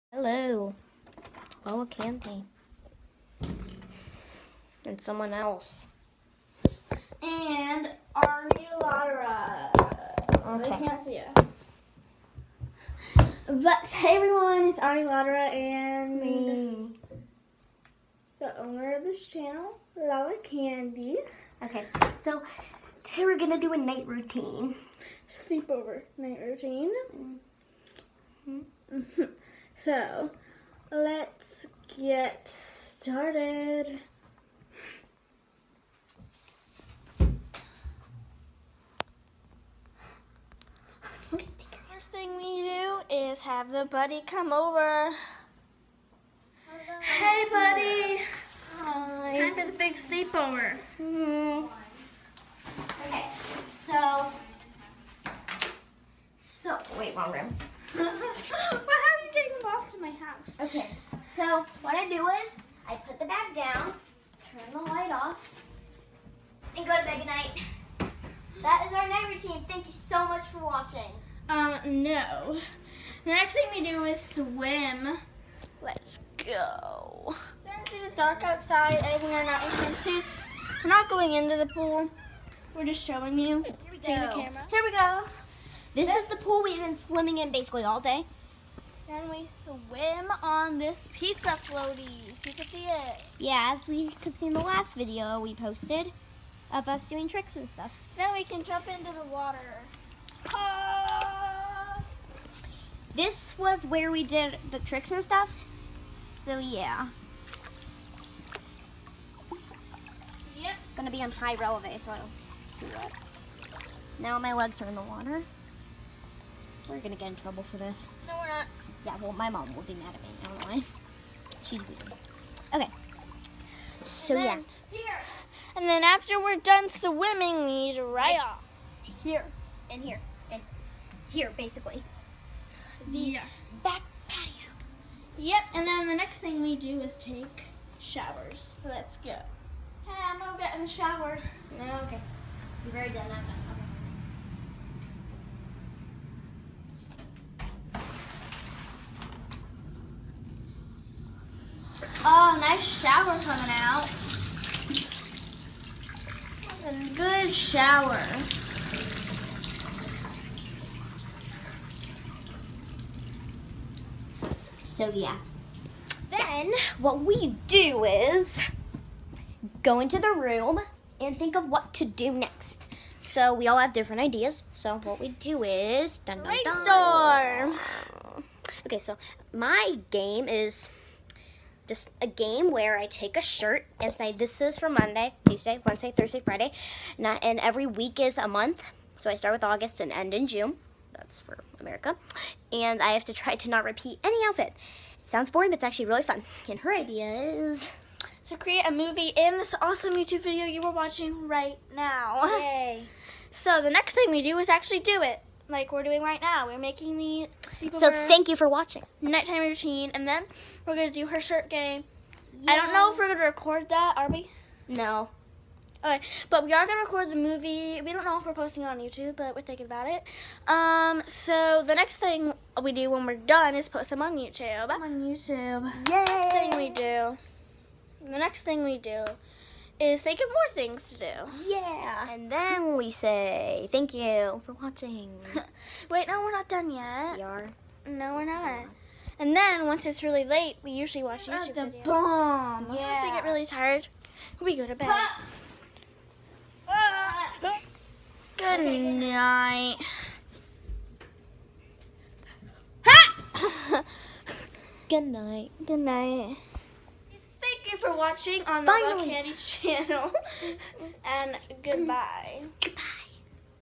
SLEEPOVER NIGHT ROUTINE!!
Me and my friend Ari Lottera's SLEEPOVER night routine!! Enjoy!!